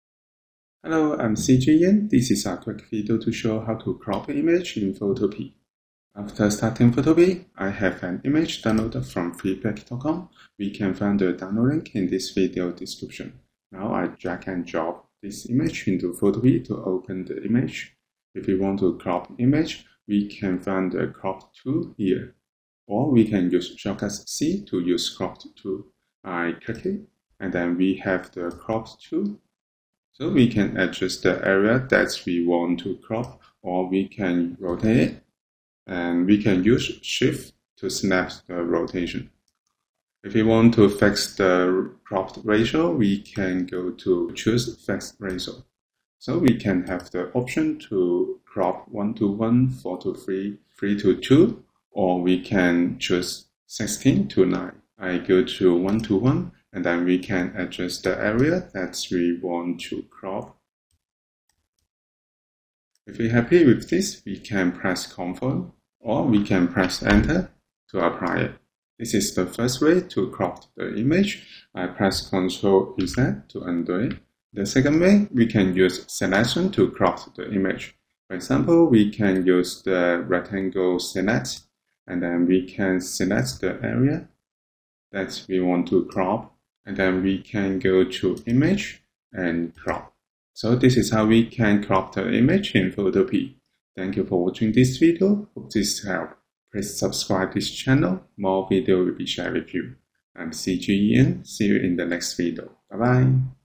How to crop image in Photopea Free Online
Tutorial video shows how to crop image in Photopea. Photopea is free online application for photo editing. We can use Photopea to crop a single image. In this tutorial, we will go through how to use Photopea to crop a specific image, we can resize image with keeping aspect ratio, and crop to selection. Steps are easy to follow for Photopea beginners. Happy sharing and I hope this helps.

Timestamp
0:00 Intro - Photopea Crop Image
0:15 Open image in Photopea
0:27 Crop Tool in Photopea
0:44 Crop with fixed aspect ratio
1:20 Crop image selection